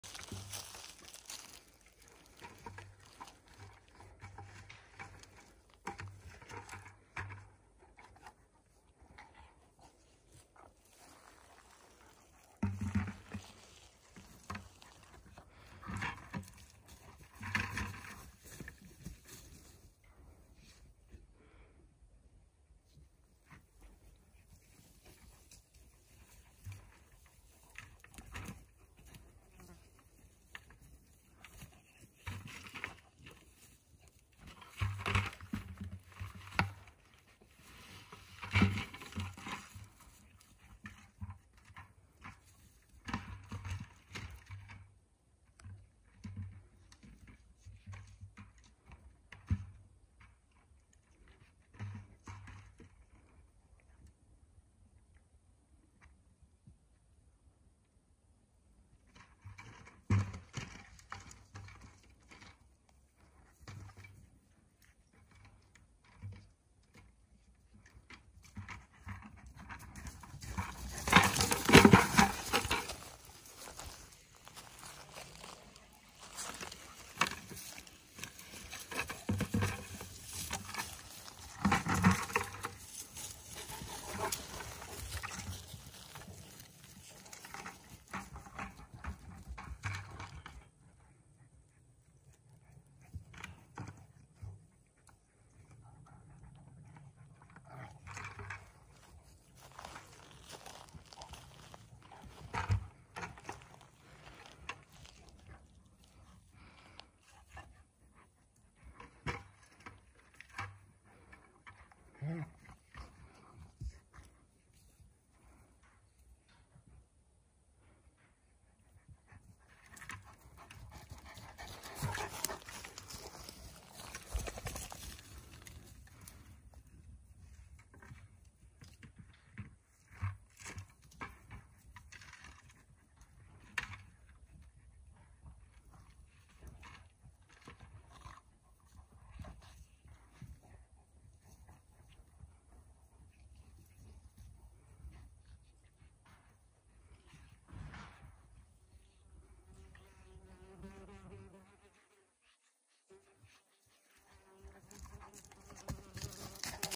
Central Asian Shepherds in the woods , playing with empty bucket!
These two always have some things to do ! Still hot in Oregon .